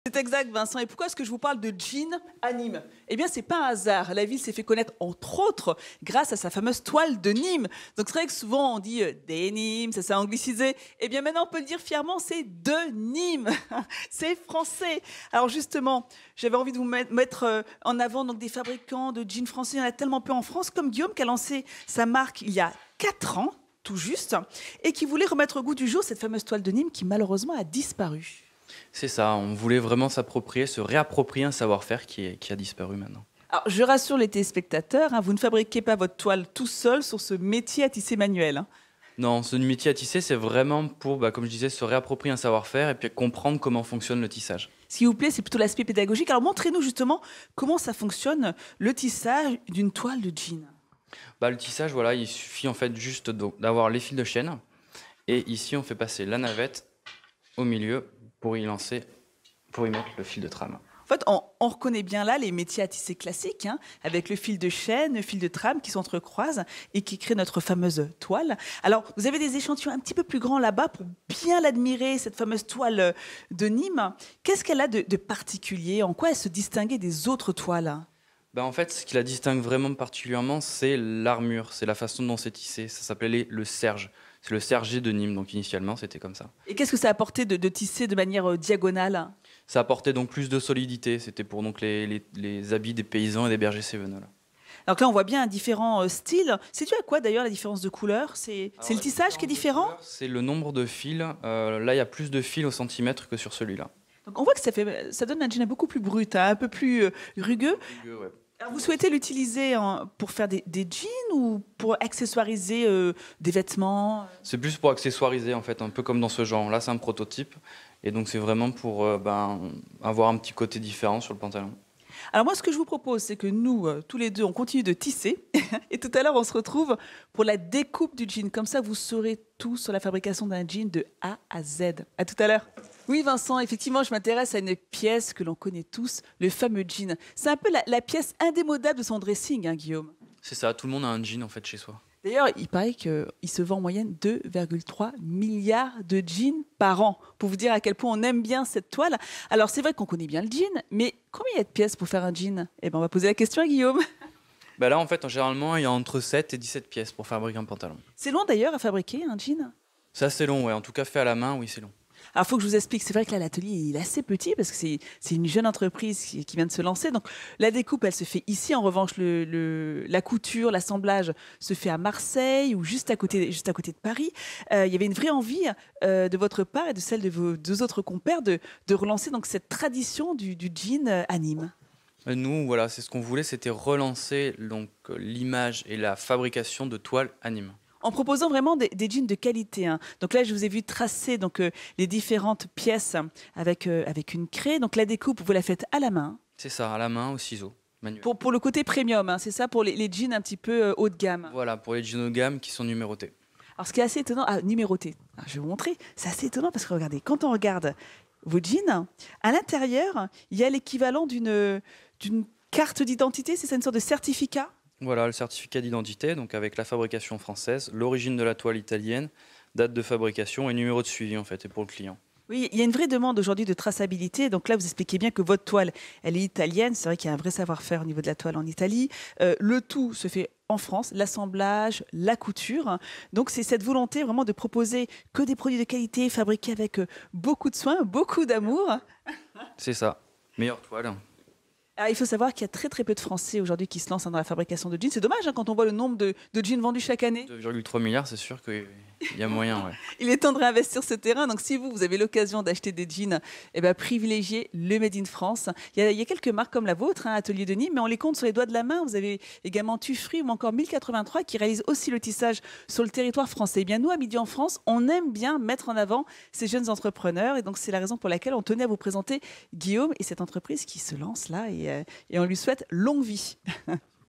Le jean continue à tisser son histoire à Nîmes
Made In France : Nathalie Schraen-Guirma vous dévoile les secrets de fabrication du jean dans un atelier de Nîmes, en présence de Guillaume Sagot, créateur de l'atelier.